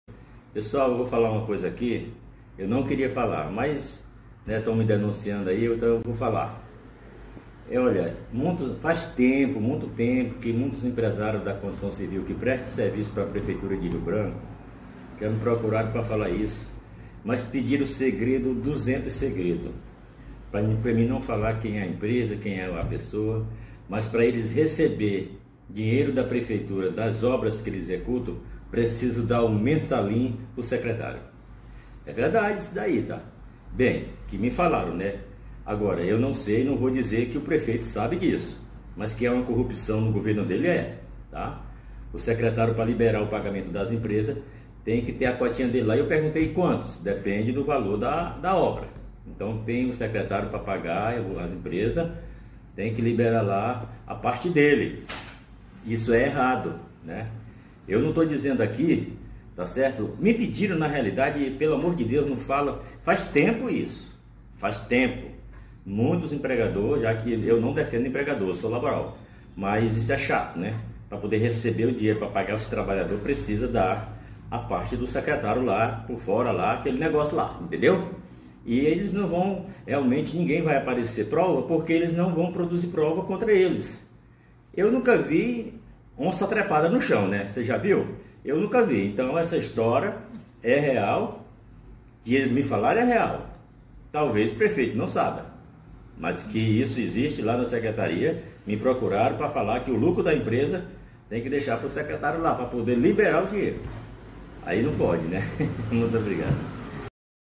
Sindicalista diz que empresários da construção civil são pressionados a pagar “mensalinho”.
O presidente do Sindicato dos Trabalhadores da Construção Civil do Acre, José Adelmar Moura de Assis, conhecido como Dema Assis, usou as redes sociais nesta sexta-feira, 27, para fazer uma grave denúncia sobre um suposto esquema de corrupção envolvendo um secretário da Prefeitura de Rio Branco.

Em um vídeo, Dema relatou que empresários da construção civil, que prestam serviços para a prefeitura, são pressionados a pagar um “mensalinho” para garantir a liberação dos pagamentos pelas obras executadas. Ele não cita o nome do secretário envolvido na suposta prática criminosa.

“Olha, faz tempo, muito tempo, que um dos empresários da construção civil que presta serviço para a Prefeitura de Branco me procurou para falar sobre isso. Ele pediu segredo, muitos segredos, para não revelar o nome da empresa ou da pessoa envolvida. Mas ele disse que, para receber o dinheiro da Prefeitura pelas obras que executa, precisa pagar um mensalinho para o secretário. Isso é verdade, tá? Foi o que me falaram.”, afirmou.

Segundo o relato do sindicalista, o valor do pagamento extra depende do montante da obra em questão. “O secretário tem que receber a parte dele para liberar o dinheiro. Eu perguntei quantos, e me disseram que depende do valor da obra”, explicou.

Dema frisou que, apesar de não defender os empregadores, considera a situação “chata”, pois impacta diretamente o pagamento dos trabalhadores. ”Eu não estou dizendo aqui que isso está certo. Me pediram, na realidade, pelo amor de Deus, para não falar. Faz tempo, faz muito tempo que isso acontece. Muitos empregadores, mesmo que eu não defenda o empregador, eu sou laboral, me disseram que isso é chato. Para poder receber o dinheiro e pagar os trabalhadores precisam dar a parte do secretário por fora.”, afirmou.

O presidente também destacou que dificilmente haverá provas concretas do esquema, uma vez que os envolvidos não produziriam evidências contra si mesmos. “Ninguém vai aparecer com prova, porque eles não vão produzir prova contra eles. Eu nunca vi onça trepada no chão, né? Você já viu?”, ironizou.